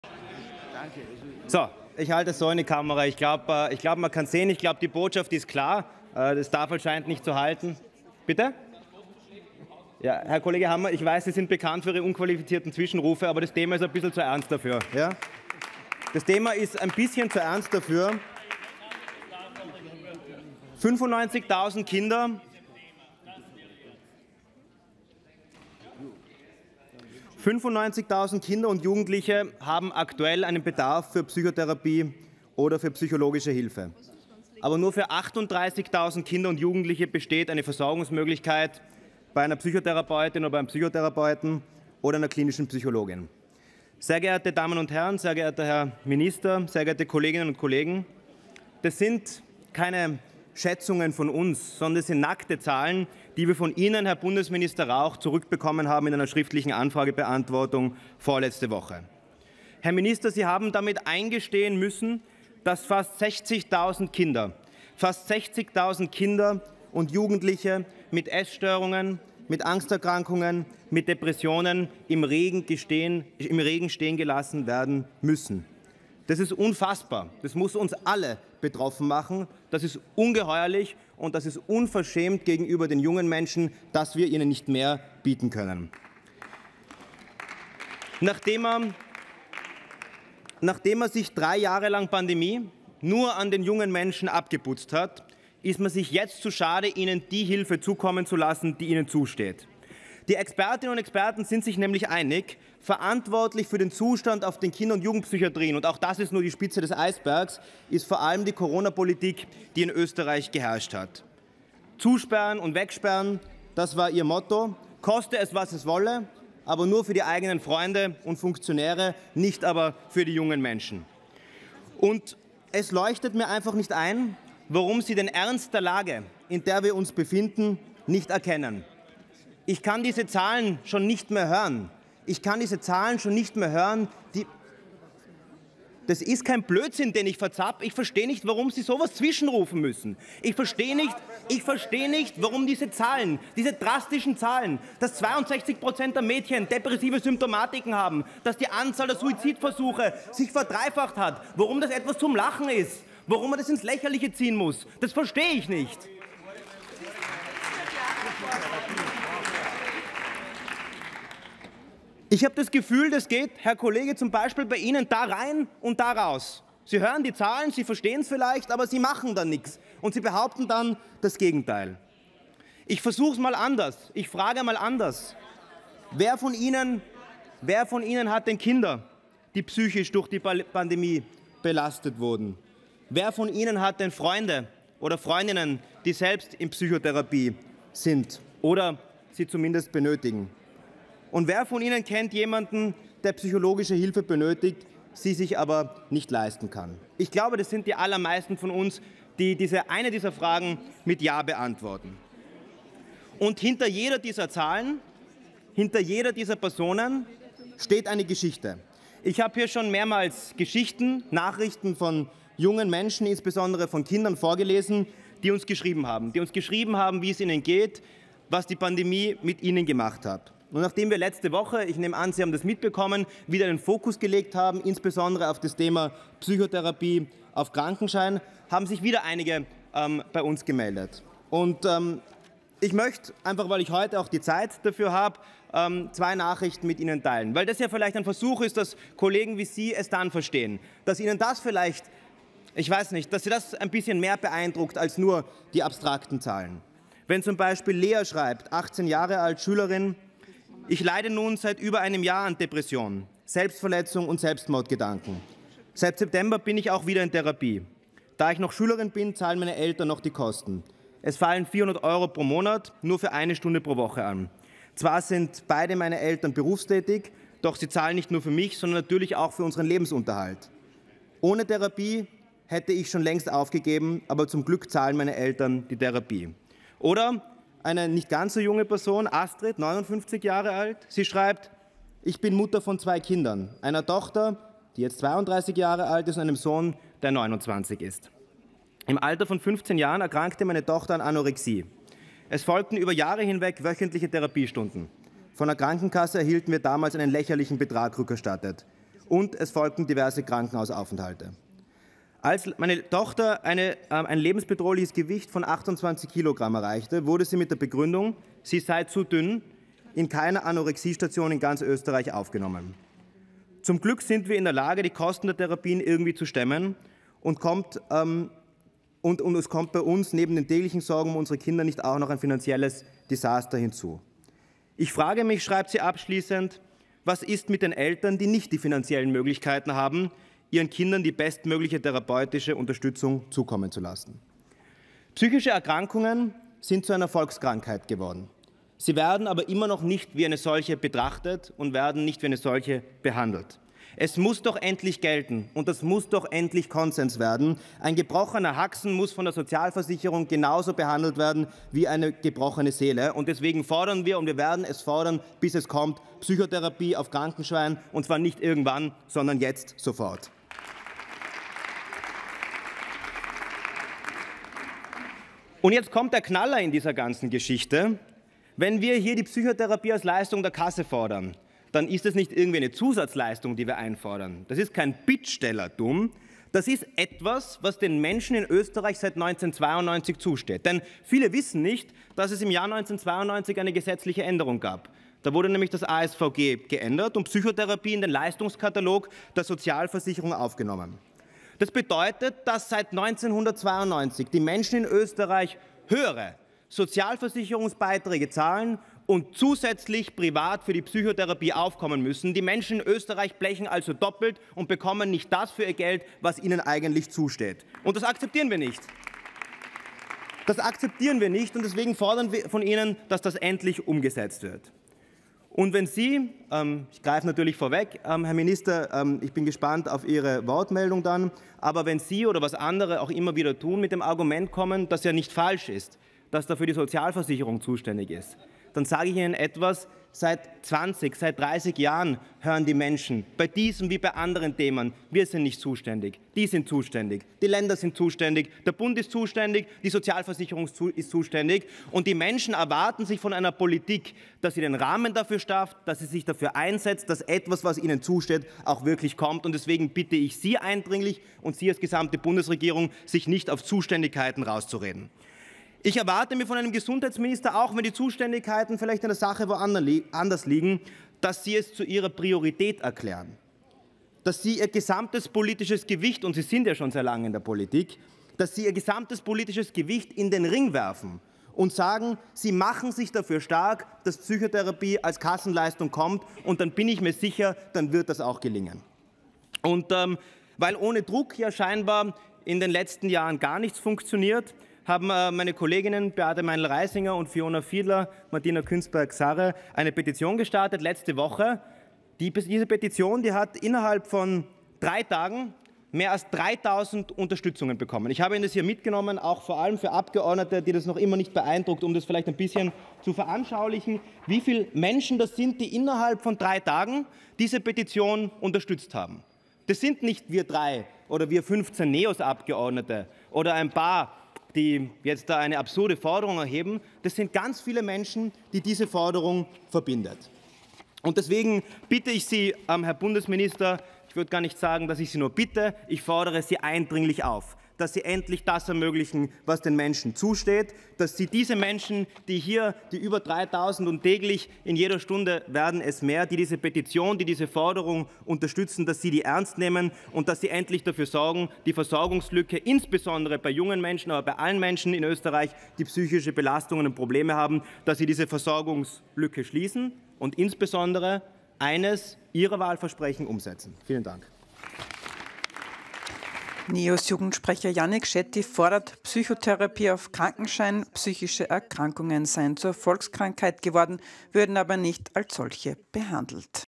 Psychotherapie auf Krankenkasse - JETZT - Yannick Shetty
Rede im Österreichischen Nationalrat am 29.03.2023 von Abgeordneten Yannick Shetty (NEOS).

Yannick Shetty hat in seiner Rede darauf hingewiesen, dass es in Österreich fast 60.000 Kinder und Jugendliche mit Essstörungen, Angsterkrankungen und Depressionen gibt, die keine ausreichende psychotherapeutische oder psychologische Hilfe erhalten. Laut einer Anfragebeantwortung von Bundesminister Rauch haben 95.000 Kinder und Jugendliche einen akuten Bedarf für Psychotherapie oder psychologische Hilfe, aber nur für 38.000 besteht eine Versorgungsmöglichkeit bei einem Psychotherapeuten oder einer klinischen Psychologin.

Shetty kritisiert die Coronapolitik der Regierung und sagt, dass die Verantwortung für den Zustand auf den Kinder- und Jugendpsychiatrien allen voran bei der Regierung liegt. Warum erkennt die Regierung den Ernst der Lage nicht?